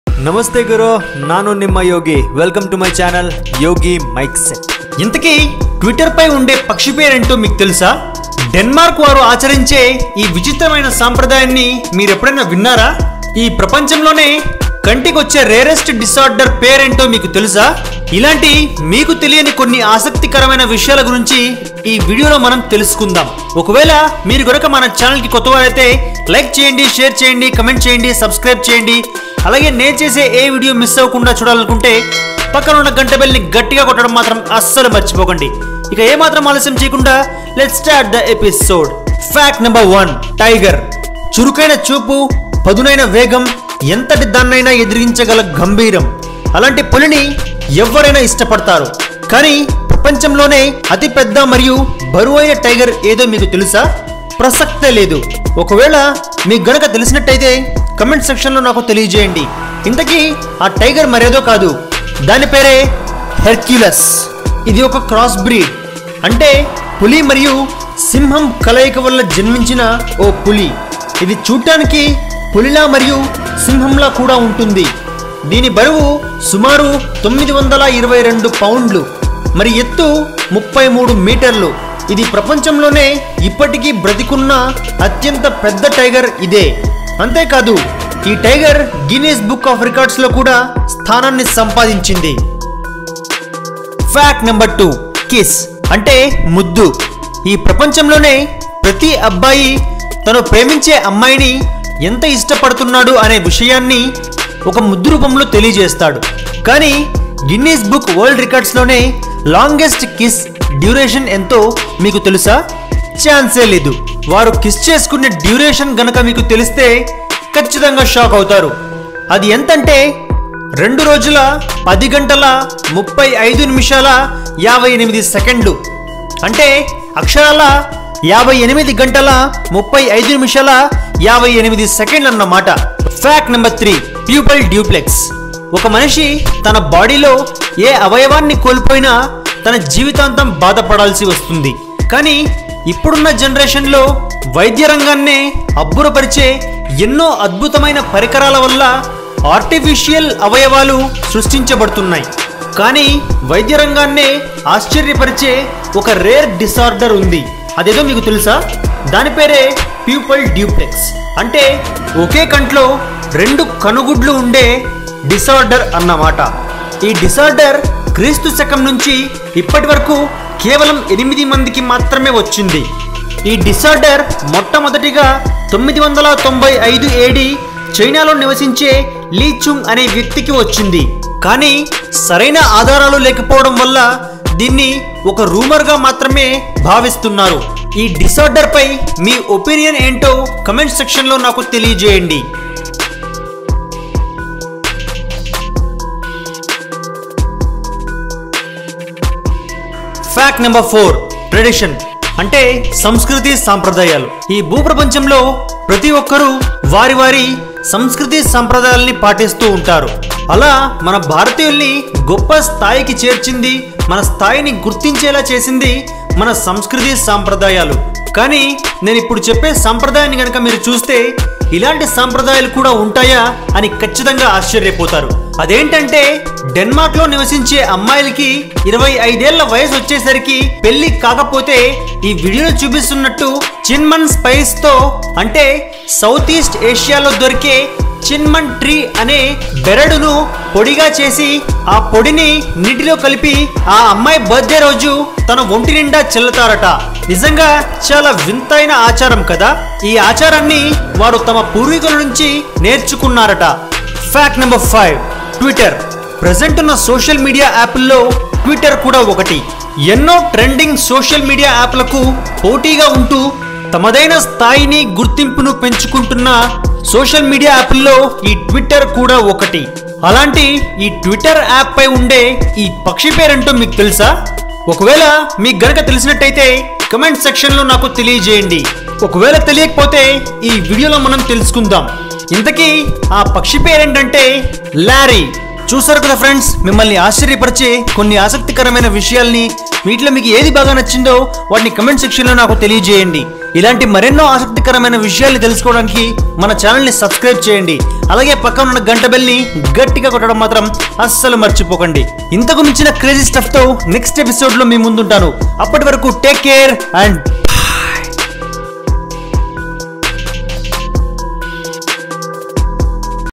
World's Longest Kiss Duration And More Intresting Facts || In Telugu
Interesting Facts In Telugu | Telugu Facts | Yogi Mikeset In Telugu | Episode 24

Follow Yogi Mikeset